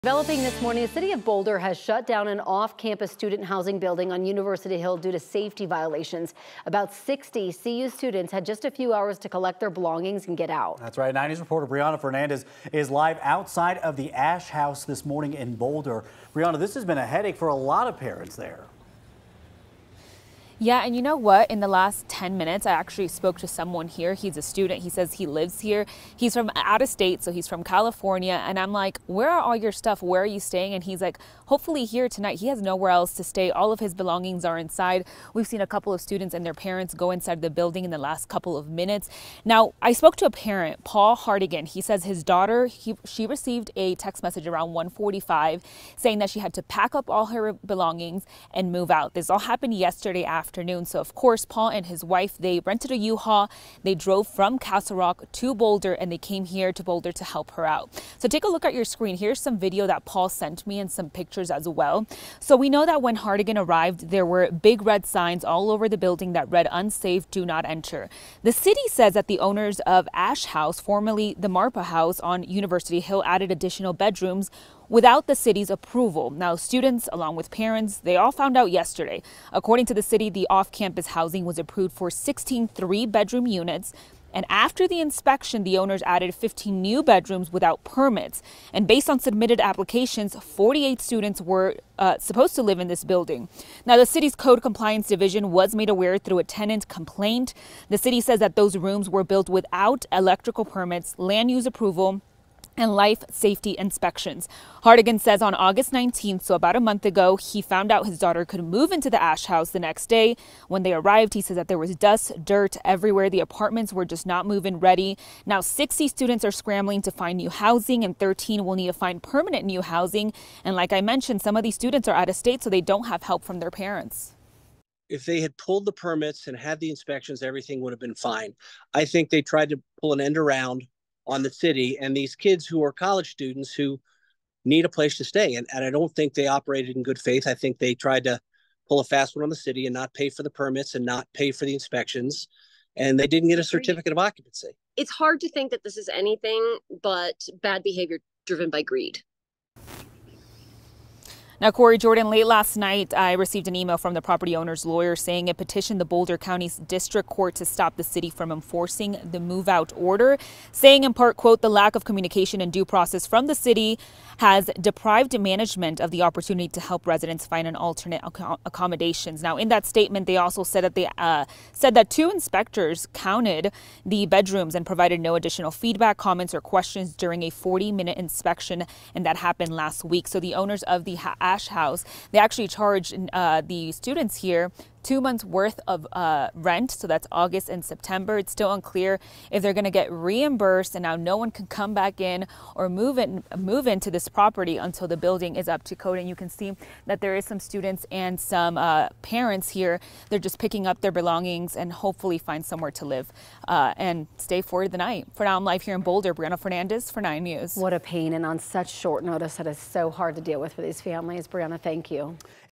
Boulder issues immediate closure notice for off-campus student housing building
The notice was issued Monday afternoon. Last week, the city discovered the property owner created additional bedrooms without city approval.

9NEWS (KUSA) is located in Denver, Colorado.